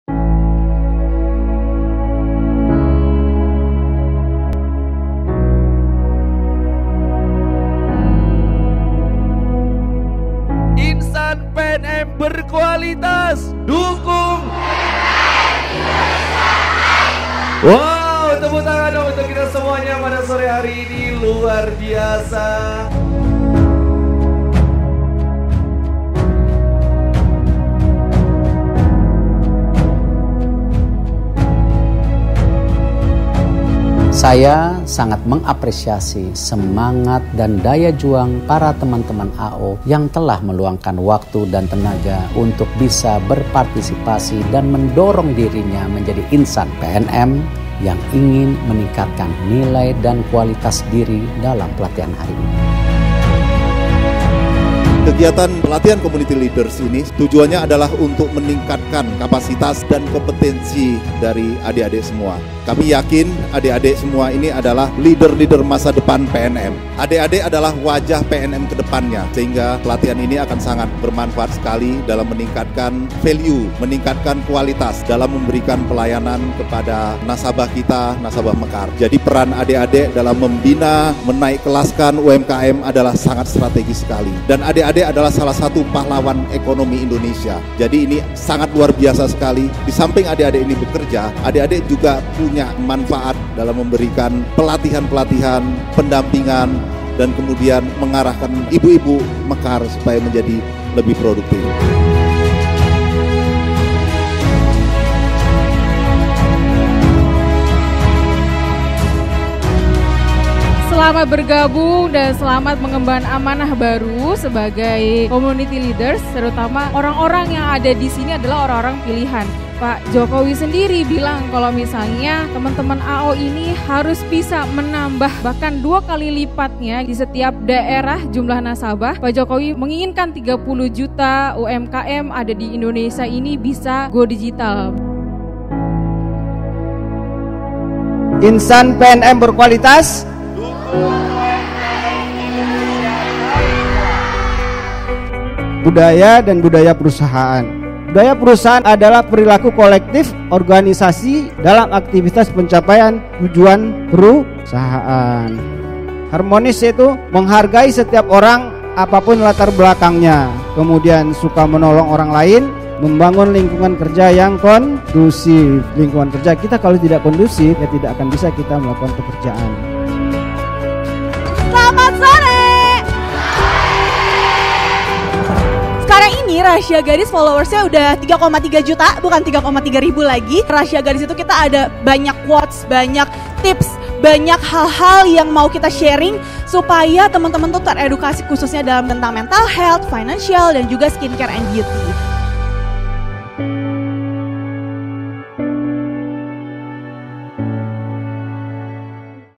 Pelatihan Community Leaders Pekanbaru Cetak Pahlawan Indonesia Baru
Pelatihan Community Leaders Mendorong Insan PNM Untuk Mendukung UMKM Naik Kelas di Pekanbaru

SobatPNM pada hari Jumat, 25 November 2022, PNM berkolaborasi dengan Karya Digital Madani (KDM) melalui program Pengembangan Kapasitas Usaha (PKU) menyelenggarakan Pelatihan Community Leaders dengan tema “Insan PNM Berkualitas, Dukung UMKM Indonesia Naik Kelas” yang diselenggarakan di Prime Park, Pekanbaru.

Kegiatan ini juga dihadiri oleh 500 peserta yang merupakan pendamping nasabah PNM Mekaar di daerah Pekanbaru. Pelatihan yang berkolaborasi antara PNM PKU dan KDM ini memberikan beberapa program dan pelatihan yaitu materi pelatihan community leaders, budaya perusahaan dan kesehatan mental.